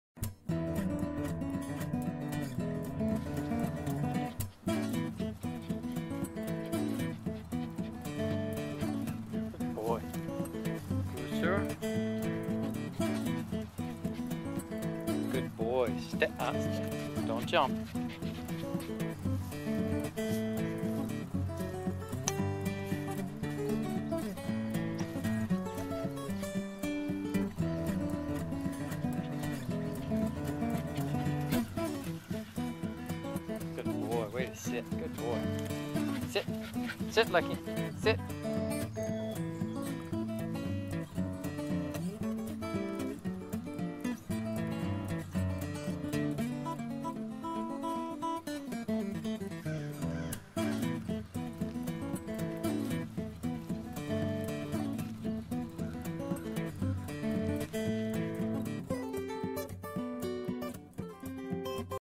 New Australian Black Lab Puppies at High Country Stables
just got two new Australian Shepard/black lab puppies. They are 8 weeks old and cute as can be!